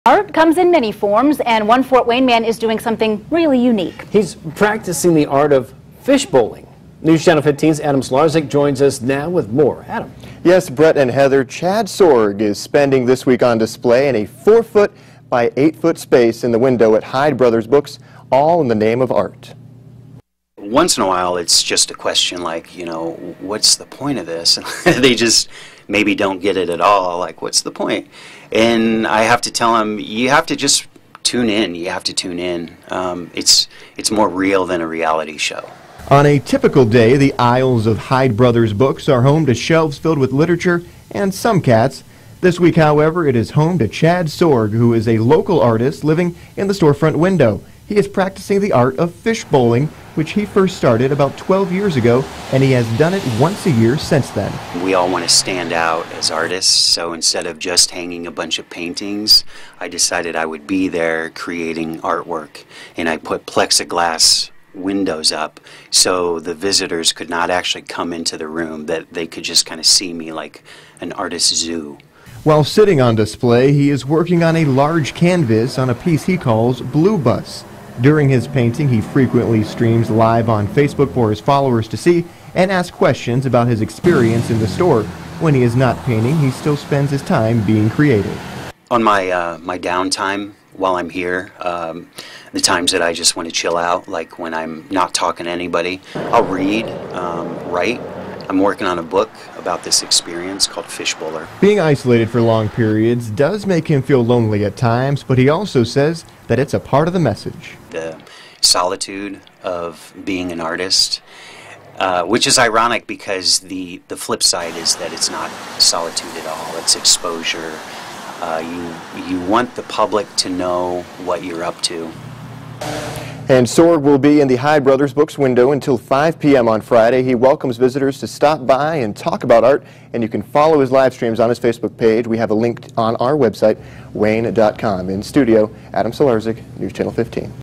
Local Artist is Fishbowling
Local Artist Living in a Window for Art